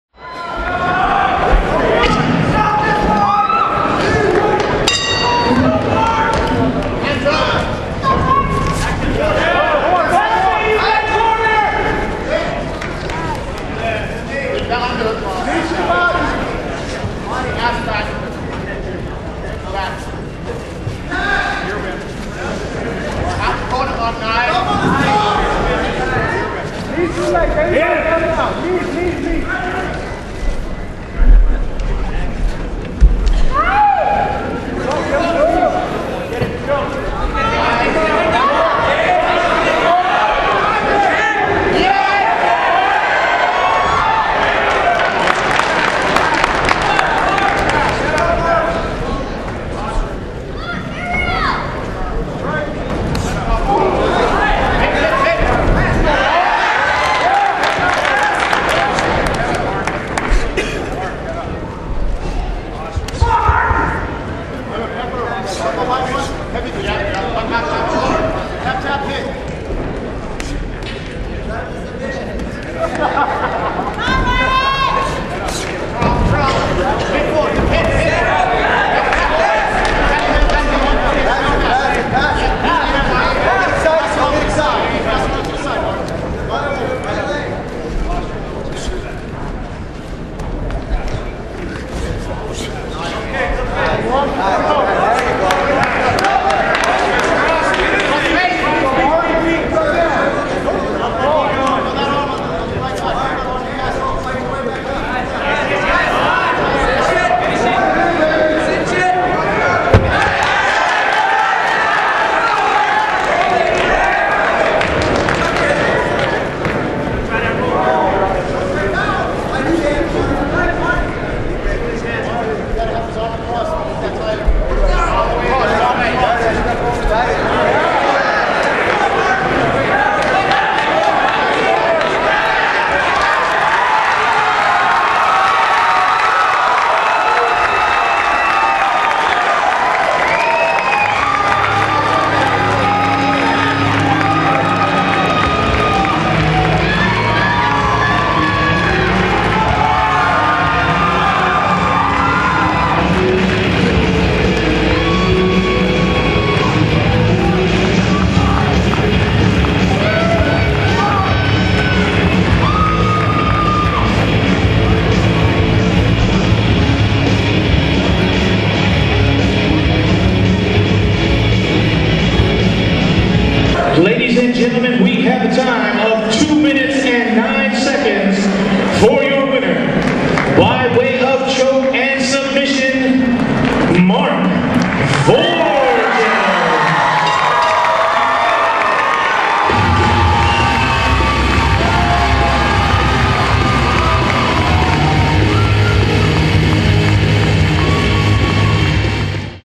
Mark Vorgeas vs. Martin "Achillies" Bautista 150lbs
Bout #7 at Long Beach Fight Night January 3, 2010.